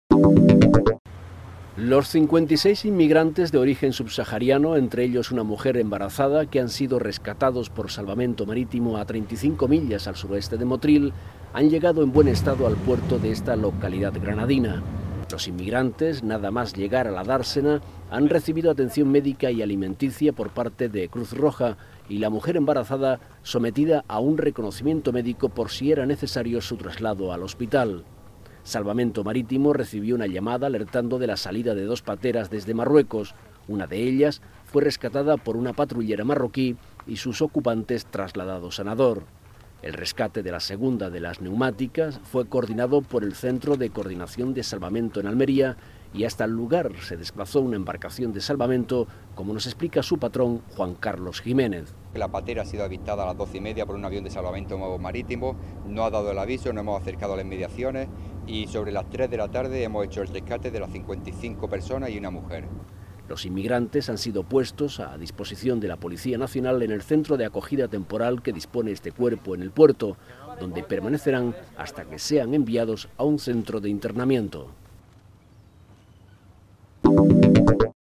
Llegan al puerto de Motril 56 inmigrantes, entre ellos una embarazada
Motril (Granada), 17 nov (EFE TV).- Los 56 inmigrantes de origen subsahariano, entre ellos una mujer embarazada, que han sido rescatados esta tarde por Salvamento marítimo a 35 millas al sursureste de Motril han llegado esta tarde en buen estado al puerto de la localidad granadina.